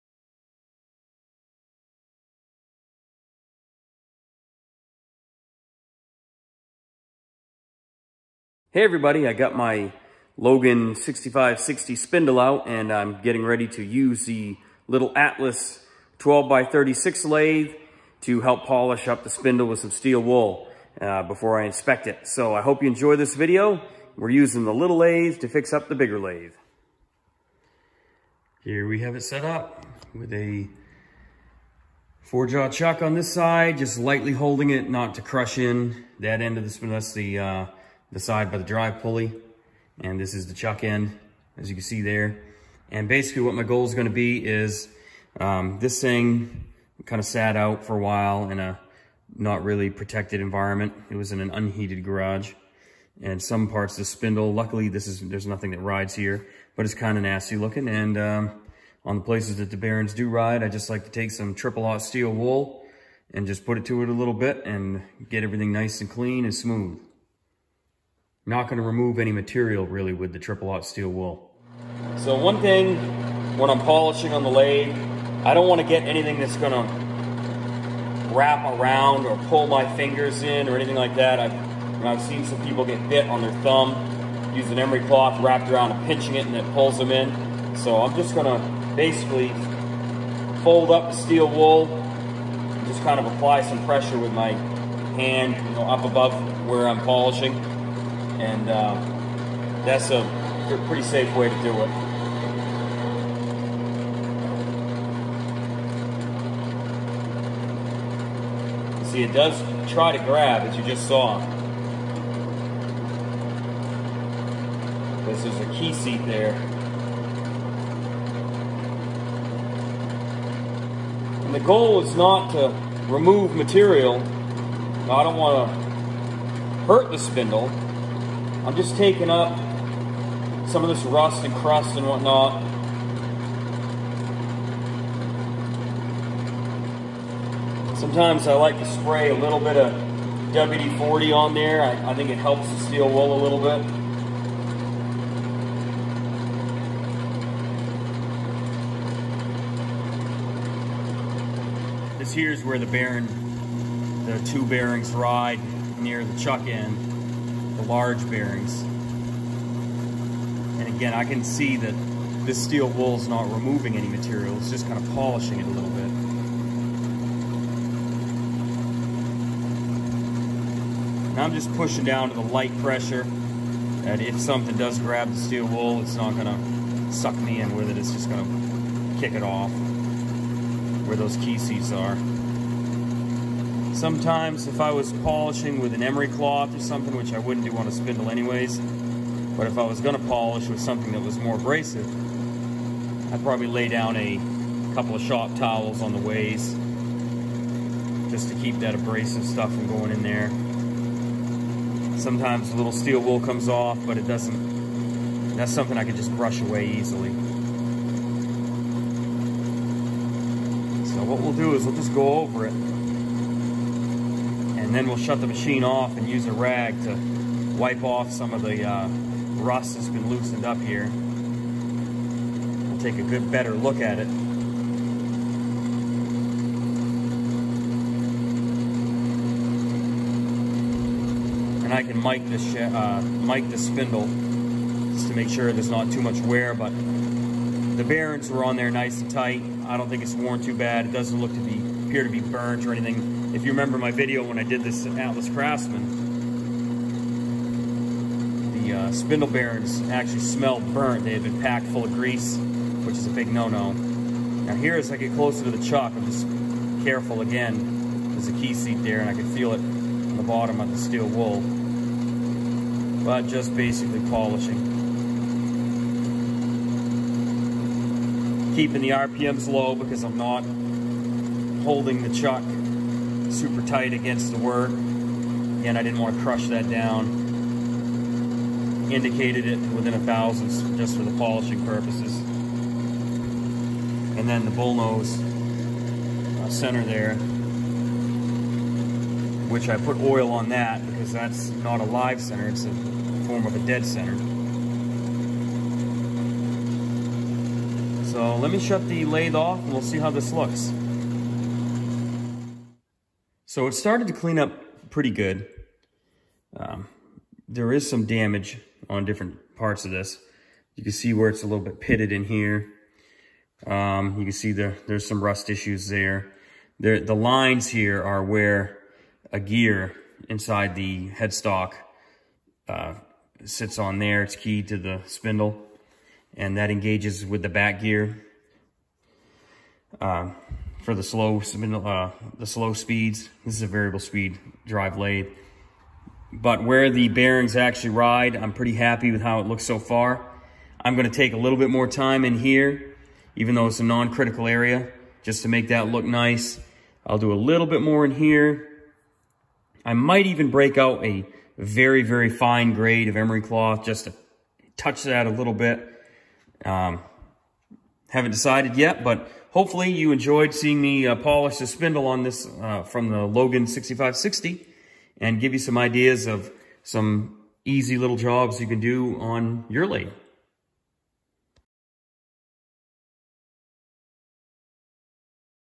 Polishing Logan 6560 Spindle in the Atlas 12
Using the Atlas Craftsman 12x36 to polish the spindle from the Logan 6560 lathe. Using one lathe to keep another one running!

Do you suffer dry or cracked hands? Have bug bites, rashes or eczema? Consider purchasing our trademarked Chapped Hide ® Plantain Salve!